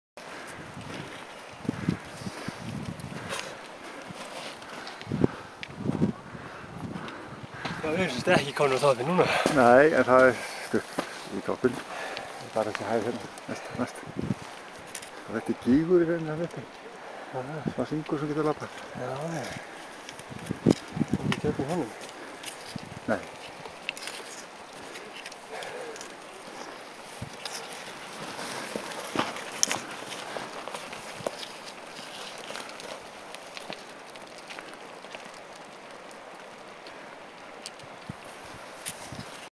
Búri in 6 peaks-hike in Tindfjallajökull - 110910
Hiking Club Toppfarar hiking six peaks in glacier Tindfjallajökull in september 2010 in a fair weather, warm and sunny in the beginning but then foggy and rainy and sunny in the end at the last two peaks. In this one we are hiking up the third peak, Búri in 1.240 m height in fog and rain with the red lava all over.